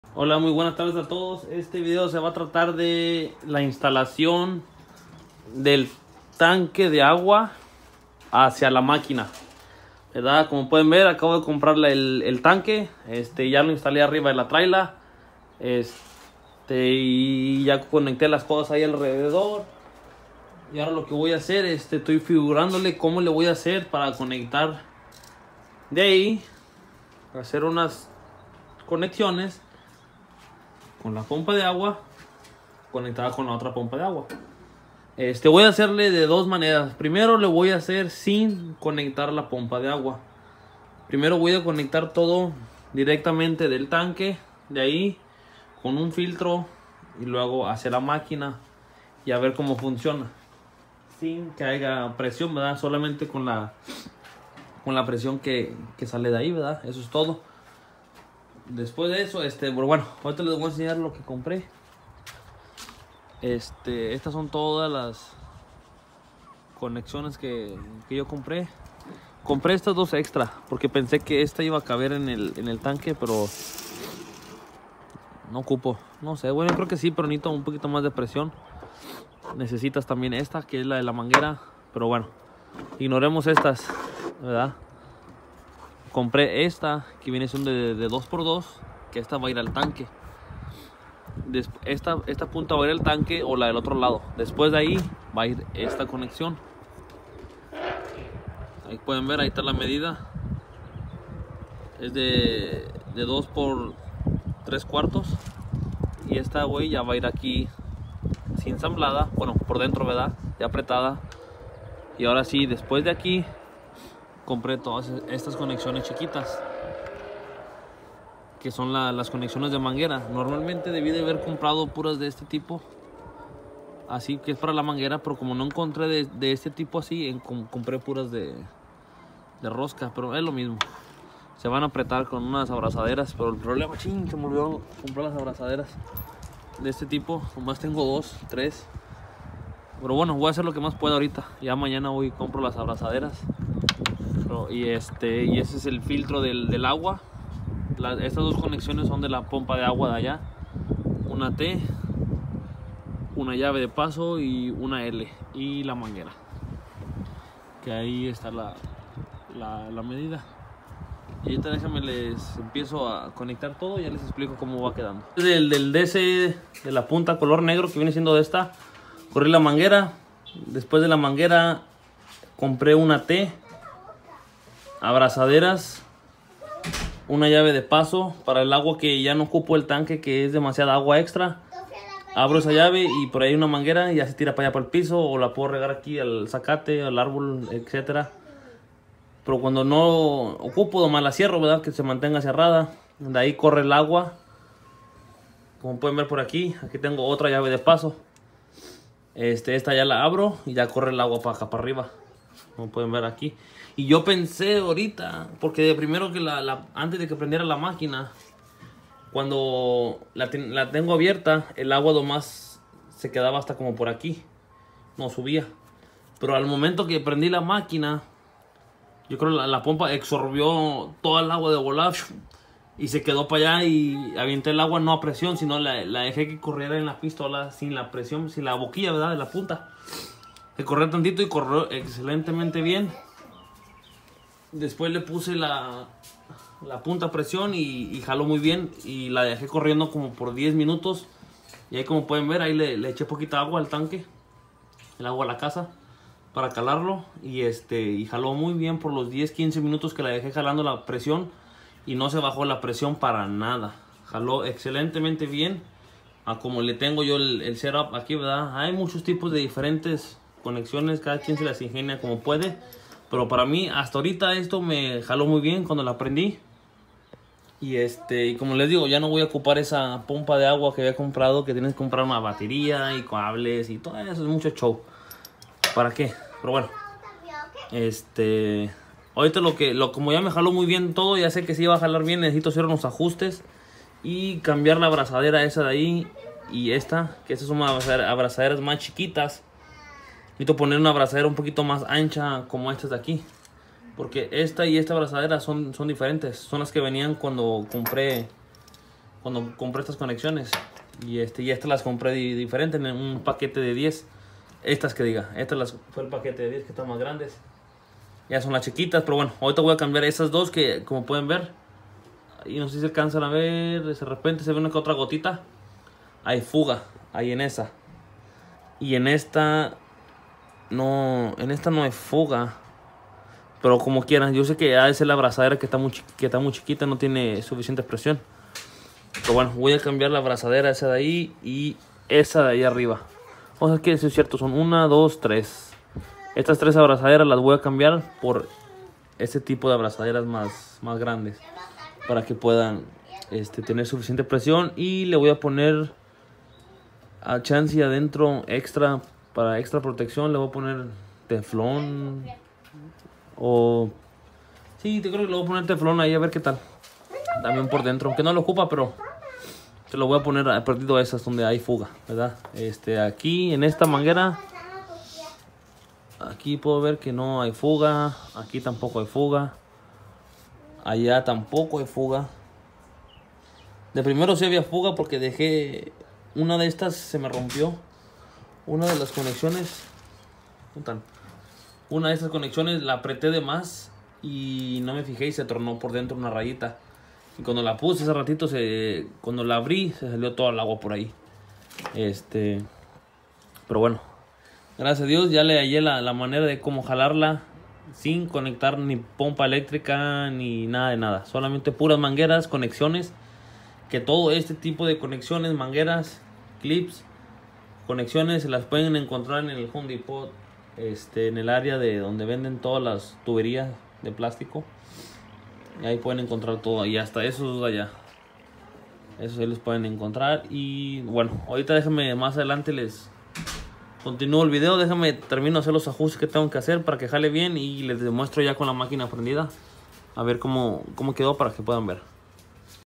#1 cómo instalar tanque de agua 💦 con máquina de presión.
Esta en proceso esto es parte 1 de la instalación pero ya casi queda espero y les ayude suerte 👍